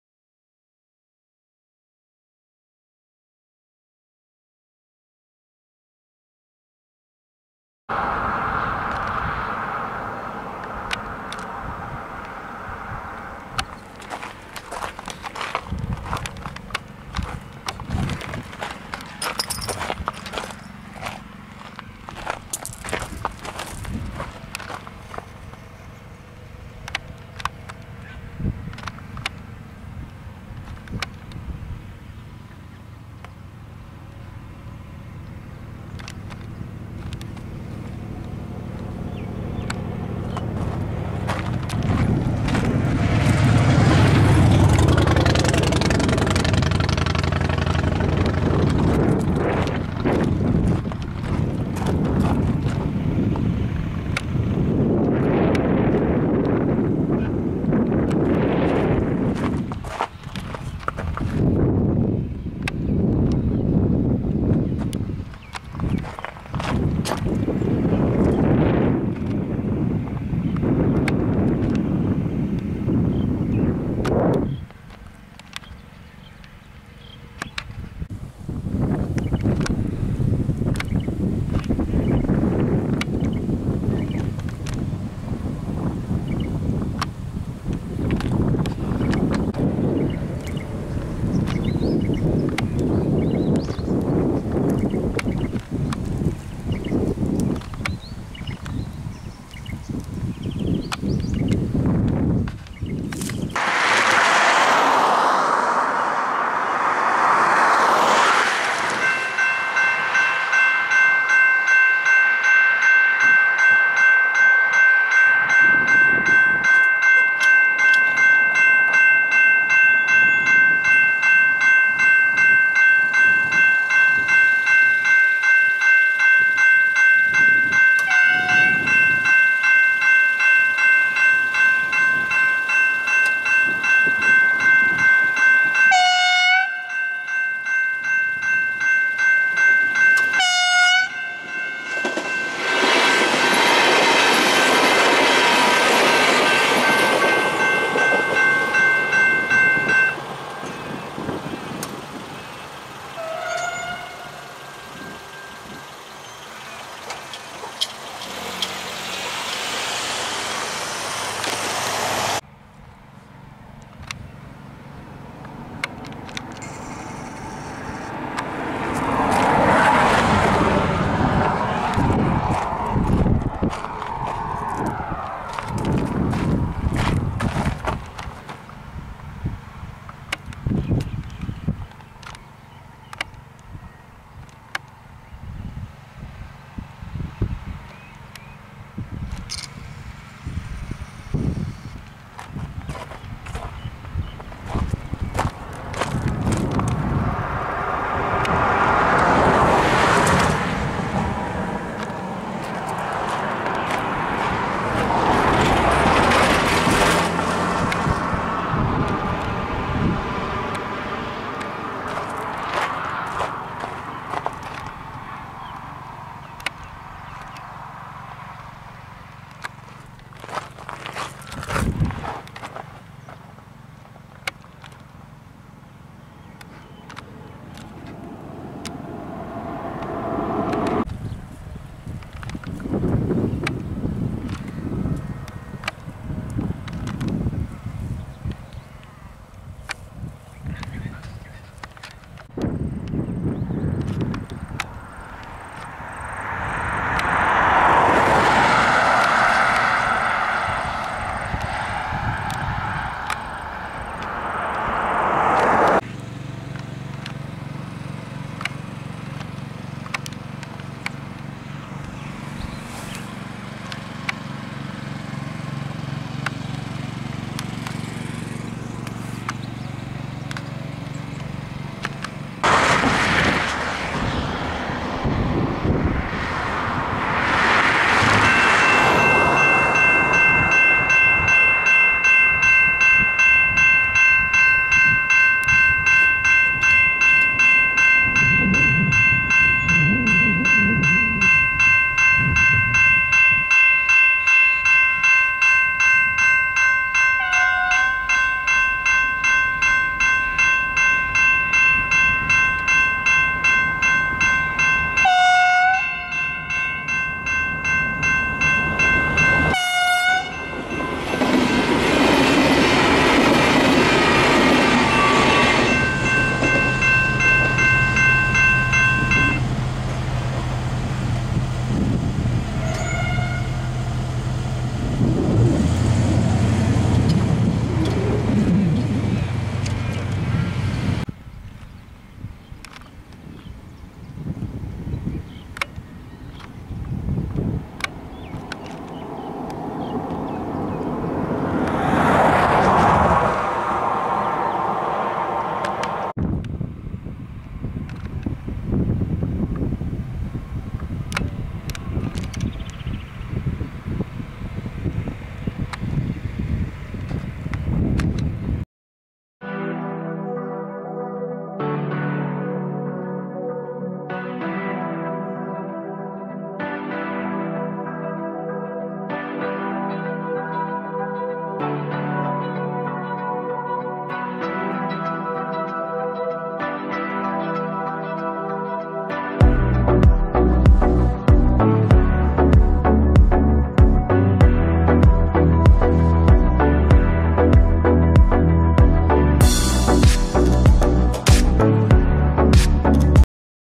Ballarat-Maryborough Road Level Crossing, Talbot, Victoria, Australia
This is the Ballarat-Maryborough Road level crossing at Talbotl, Victoria, Australia. This level crossing is located on the Maryborough railway line.

This level crossing is a gated setup and has a total of 2 signals

There is 1 track going through this level crossing which is broad gauge.

This level crossing is maintained by V/Line

The level crossing equipment consists of:

- 2 General Signals Type 2 electronic bells

- Westinghouse crossarms

- Older style Westinghouse LED lights

- Safetran Systems Corporation boom gate mechanisms

- Level crossing ahead signs with an illustration of a level crossing signal

- Level crossing ahead road markings with a rail diagram

This level crossing received boom gates in early 2010. In March 2010 the original flashing light signals were lying on the north eastern side of the level crossing

Filming date: 30th of September 2023